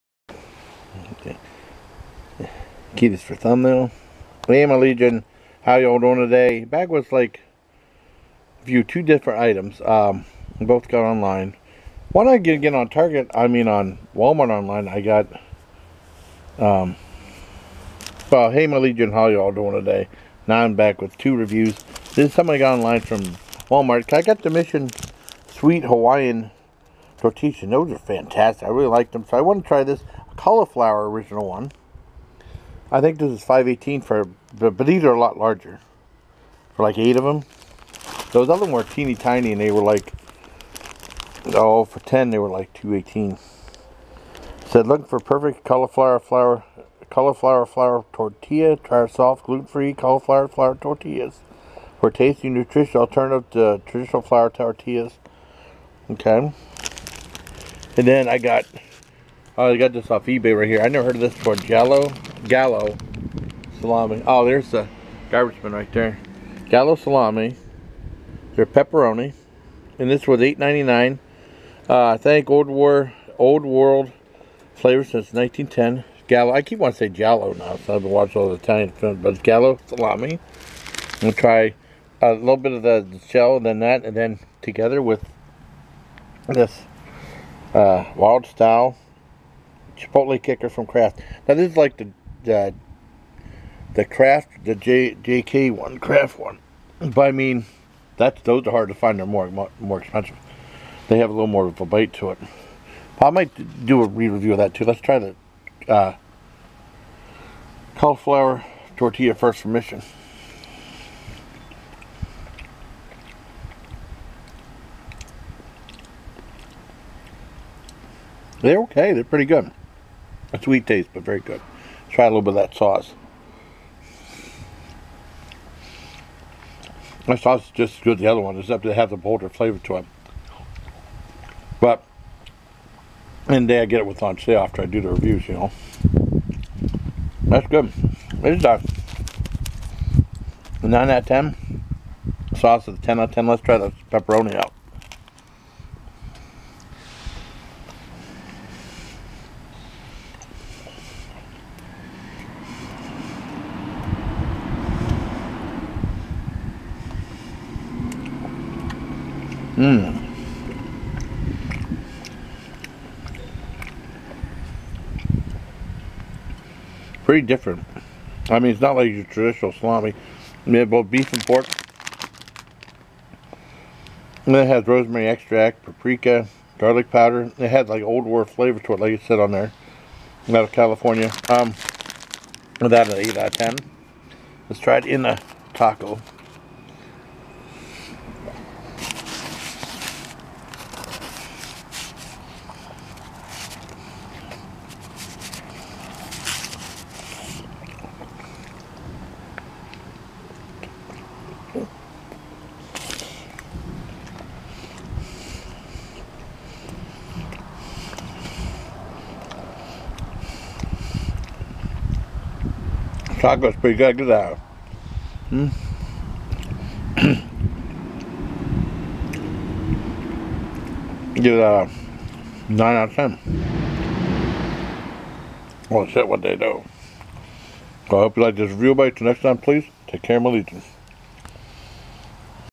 Gallo Salame Pepperoni & Mission Cauliflower Tortilla Shells
So are these 2 items any good?